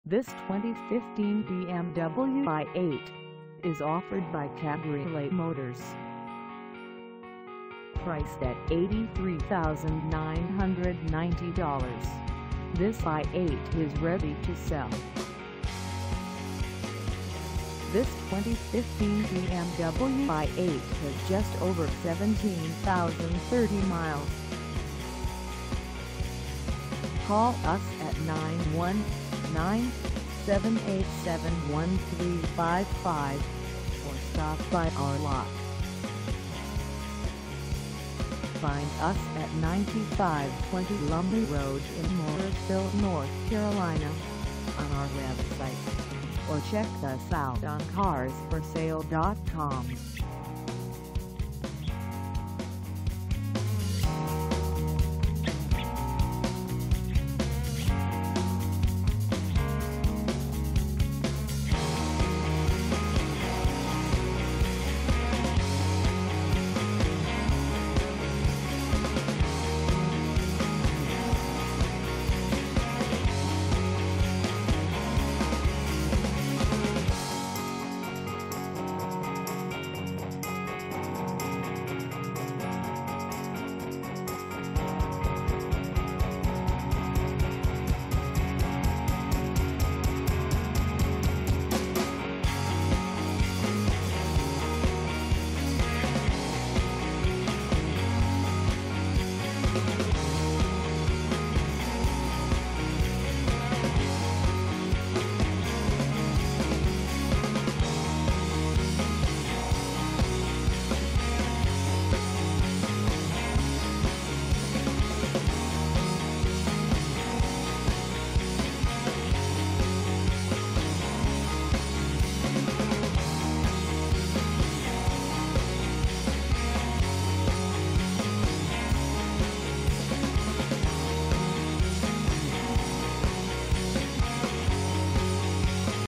2015 BMW i8 Base AWD 2dr Coupe for sale in Morrisville, NC 2
This 2015 BMW i8 Base AWD 2dr Coupe is for sale in Morrisville, NC 27560 at Cabriolet Motors.

Engine: 1.5L I3 Twin Turbo
Transmission: Automatic 6-Speed
Color: White
Mileage: 17030
Contact Cabriolet Motors 9520 Lumley Road Morrisville, NC 27560 to test drive this 2015 BMW i8 Base AWD 2dr Coupe today.